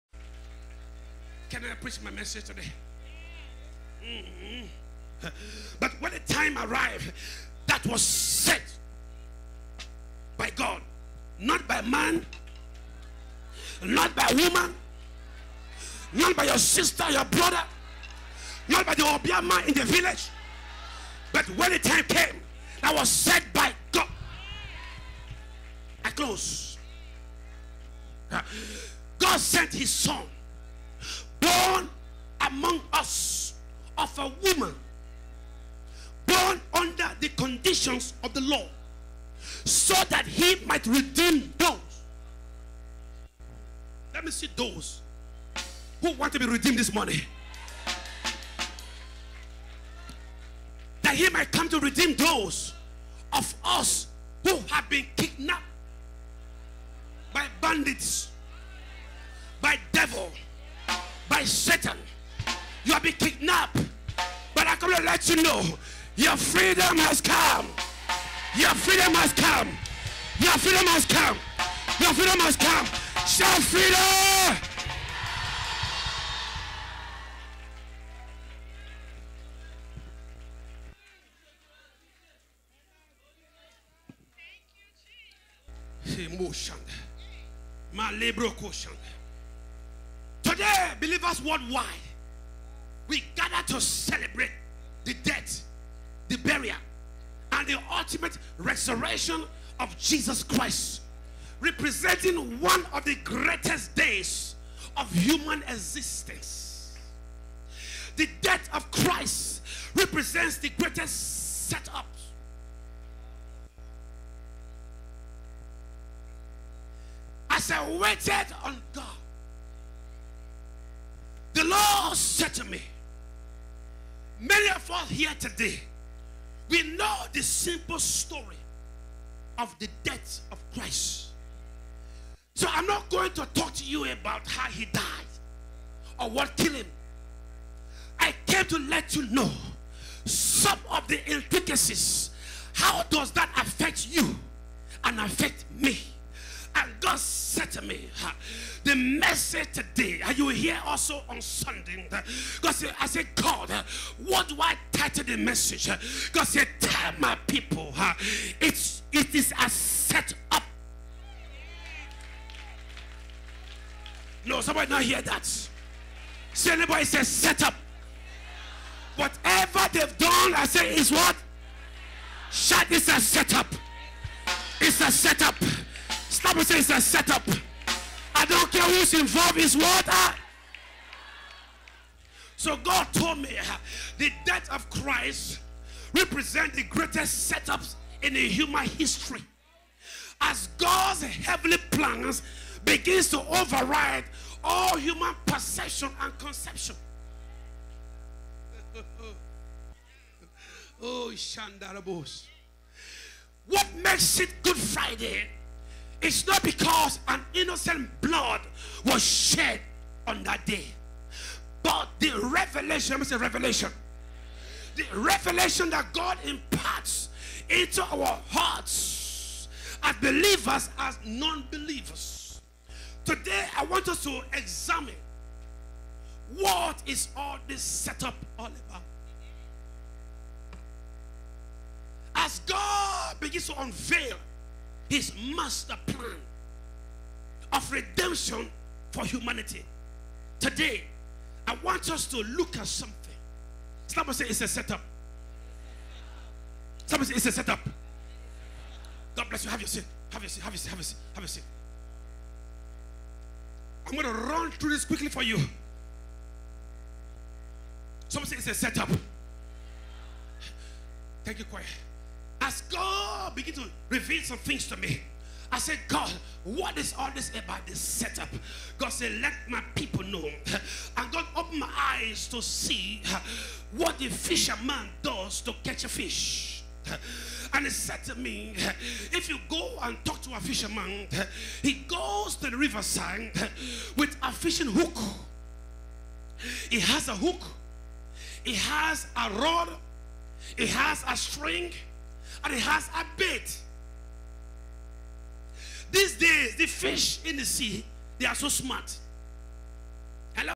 Prophet Joel Uwagboe-MESSAGE- IT'S A SET-UP
MESSAGE: IT'S A SET-UP. @ FREEDOM LIFE MINISTRIES INC. GEORGETOWN ANNUAL GOOD FRIDAY SERVICE. ON THE 25th March 2016. BY PROPHET JOEL UWAGBOE. SCENE 3